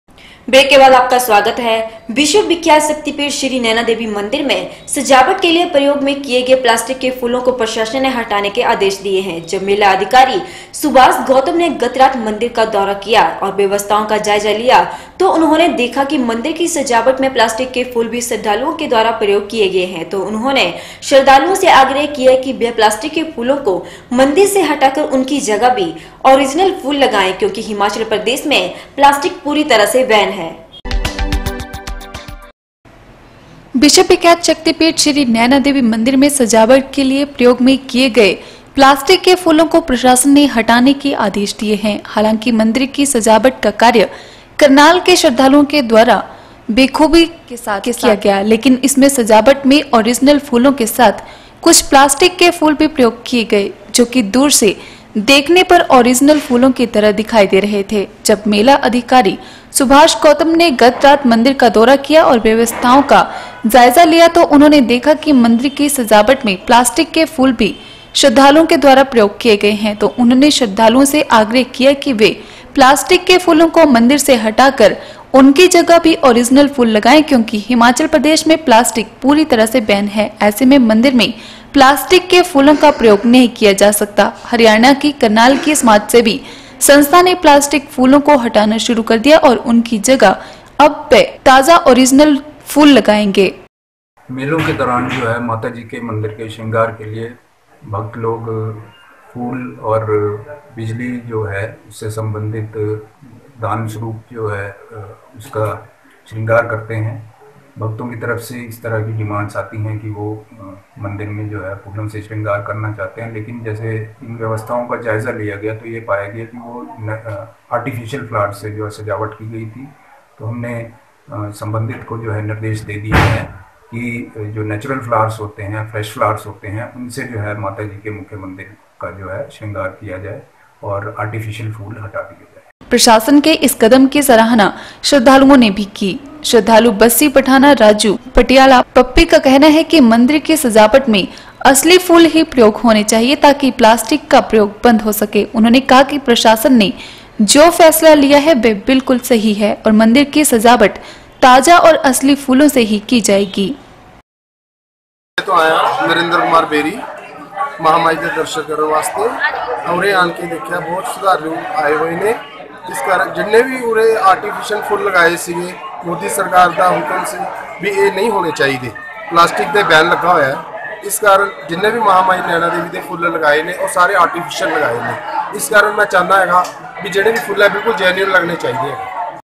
01 OCT N 9 Administration orders to remove plastic flowers
विश्व विख्यात शक्तिपीठ श्री नैना देवी मंदिर में सजावट के लिए प्रयोग में किया गए प्लास्टिक के फूलों को प्रशासन ने हटाने के आदेश दीए  है जब मेला अधिकारी सुभाष गौतम ने गत रात मंदिर का दौरा किया और व्यवस्थाओं का जायजा लिया तो उन्होंने देखा कि मंदिर की सजावट में प्लास्टिक के फूल भी श्रद्धालुओं के द्वारा प्रयोग किए गए हैं तो उन्होंने श्रद्धालुओं से आग्रह किया कि वह प्लास्टिक के फूलों को मंदिर  से हटाकर उनकी जगह भी ओरिजिनल फुल लगाएं क्योंकि हिमाचल प्रदेश में प्लास्टिक पूरी तरह से बैन है